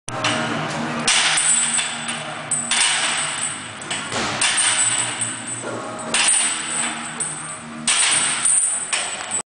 Varsity House - Max Rep Chain Push Ups
We perform this exercise as a max rep workout.  Usually performing 2-3 sets all out.  We start with 2 or 4 chains depending on the athlete's level, and strip them off one at a time as they fatigue.  Nasty!